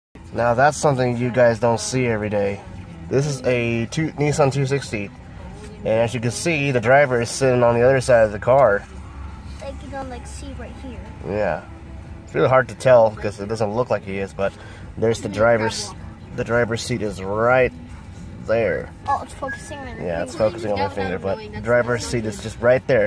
JDM Imported Nissan 260z (Legit Right Hand Drive)
This car is sold in Japan as the Fairlady Z. The Fairlady Z name also carries on to the Nissan 350z and 370z

NOTE: Importing a car that's right hand drive (mainly Japanese) is legal as long as if it meets the US regulations and passes smog check. Then registering to the DMV

1.  Foul language
2.  Spam
3.  Bullying to other individuals
4.  Racism and sexism. (No sex jokes)